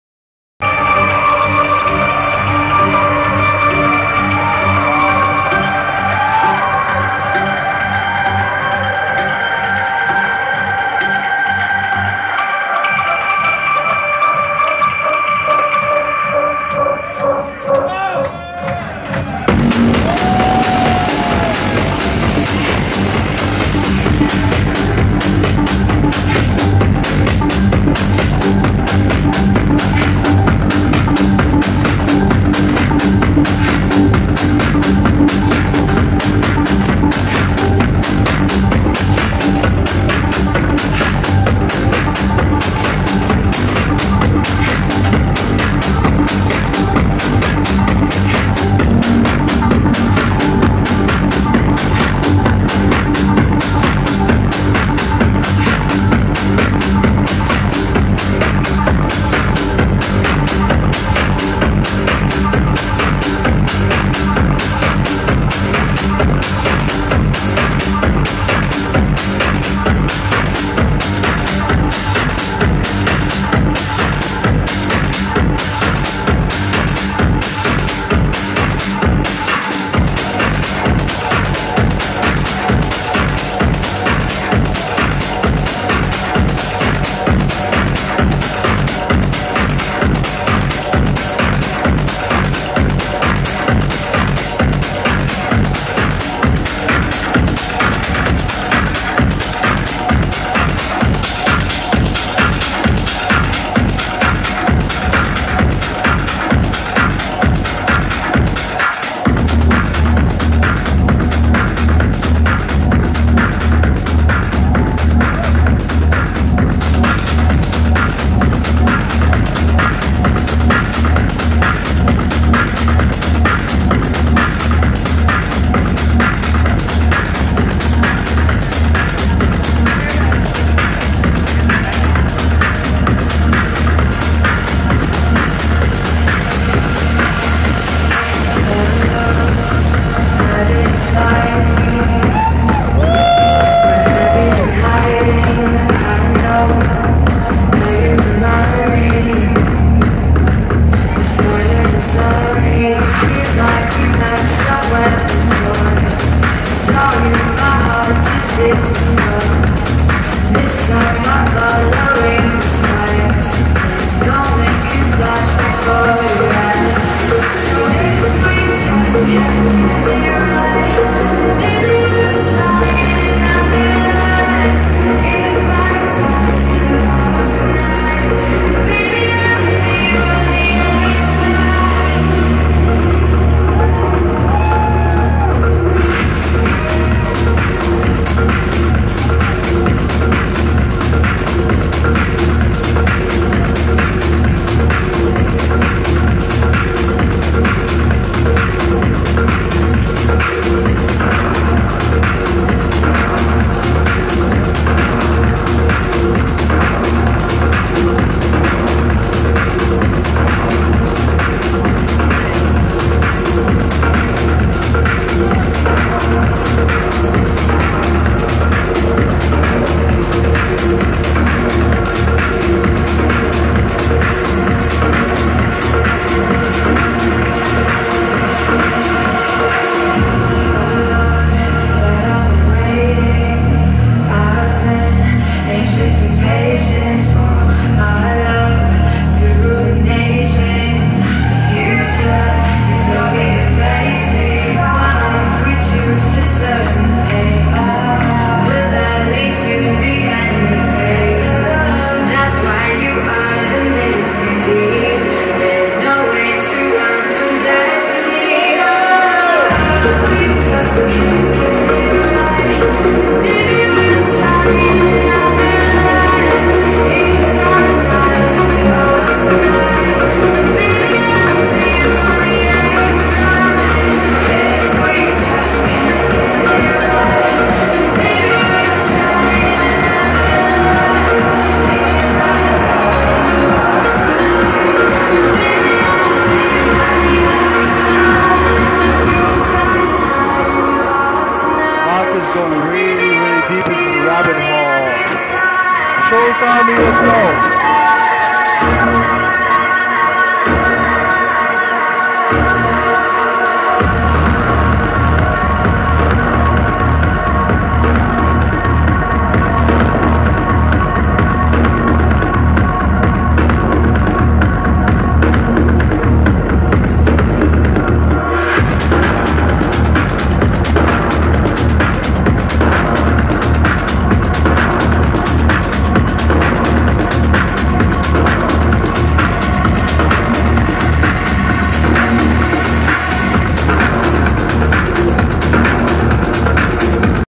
Markus Schulz destroying Space at Miami ULTRAWEEK 2016
Dark, dirty and sexy set from Space Miami by Markus Schulz